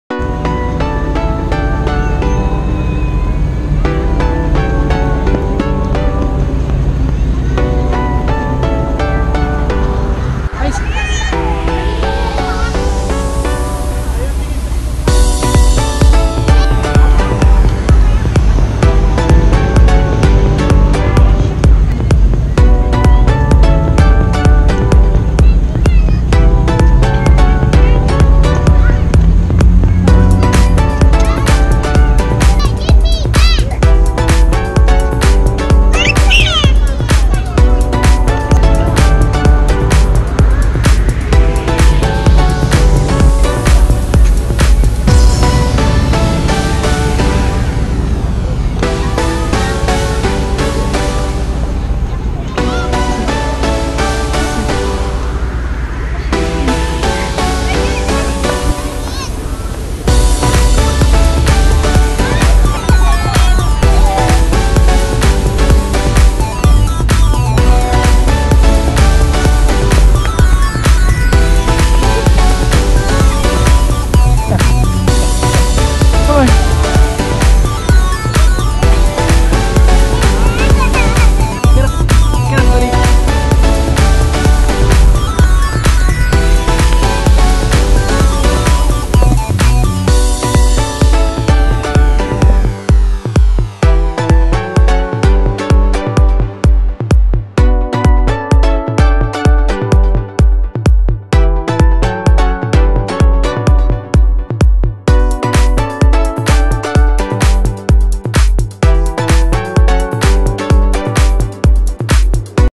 Malawaan Park with Icy: Capturing the Subic Bay Sunset | Family Bonding in Olongapo City
In this vlog, I take you to one of the best-kept secrets in Subic Bay, the Malawaan Park, where my son Luther (aka Icy) and I enjoy the stunning sunset while waiting for my mother to arrive from Las Pinas.

Malawaan Park is a recently renovated family park where everyone can now enjoy the beautiful Subic Bay sunset. The park features Bermuda grass and a kid's playground, making it a great place to bond with your family when in Olongapo City.

In this video, I capture the stunning sunset in follow mode, using my GoPro, and it was truly a breathtaking moment. Icy and I had a great time enjoying the view, and I wanted to share it with all of you.

Ever since we shot this video in early 2017, my family and I have returned to Malawaan Park several times, bringing along a picnic mat and other family members. It's a perfect rest stop if you're exploring other sights in SBMA, like Ocean Adventure.

If you're planning to visit Olongapo City, make sure to add Malawaan Park to your list of must-visit places. It's one of the first tourist destinations you can get to if you're coming from Manila, and it's a great place to spend quality time with your family.